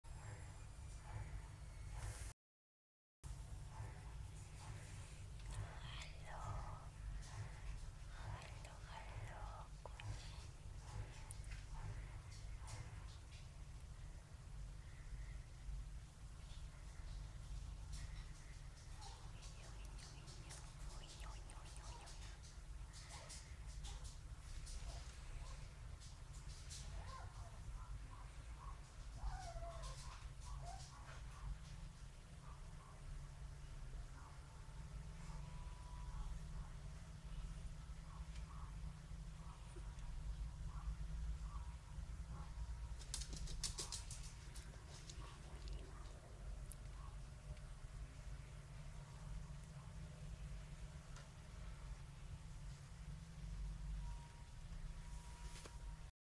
Jason vom Griffon Hof - martwy gryfonik, jak głaskać - tutorial ;)
Najedzony, śpiący snem kamiennym 3 tygodniowy szczeniak gryfonika belgijskiego.